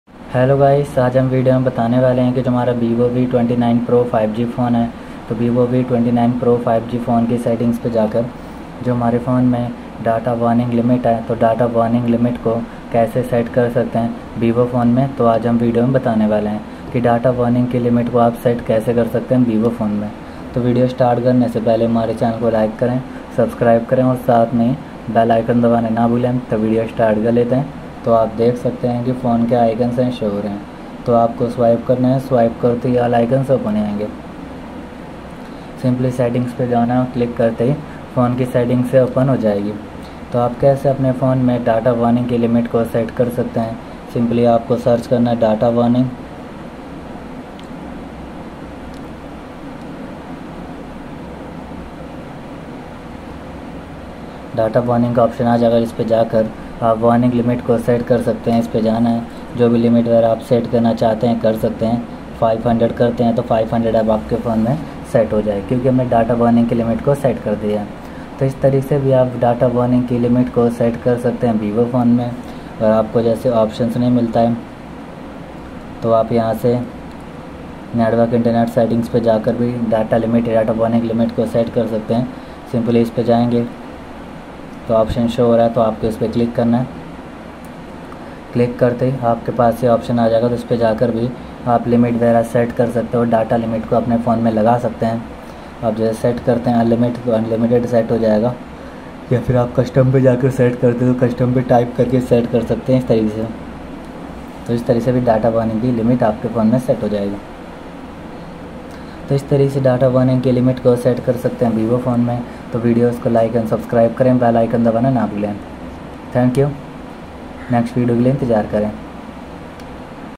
how to set data limit warning in Vivo V29 Pro 5G , Vivo V29e 5G, data limit kaise set karen
60-Inch Tripod with Bag

7 Socket 6A Extension Board

multiplug Extension